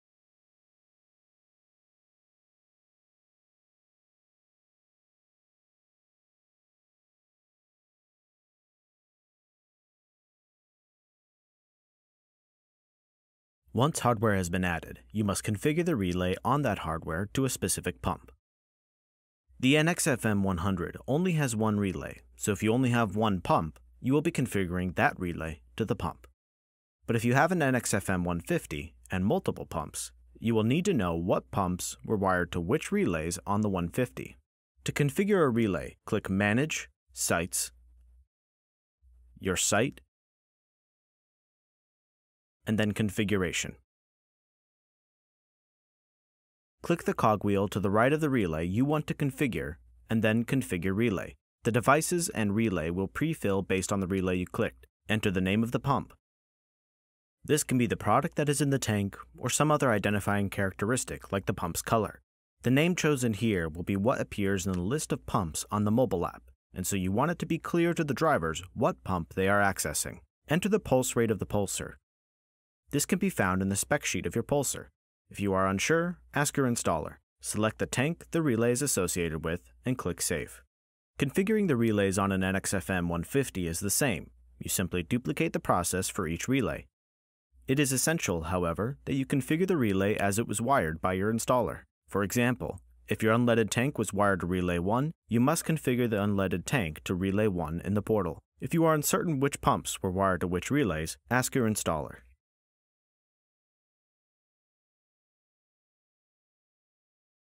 Fill-Rite FMS Web Portal Training: Configure Relays
This video demonstrates how to configure relays using the Fill-Rite FMS website portal.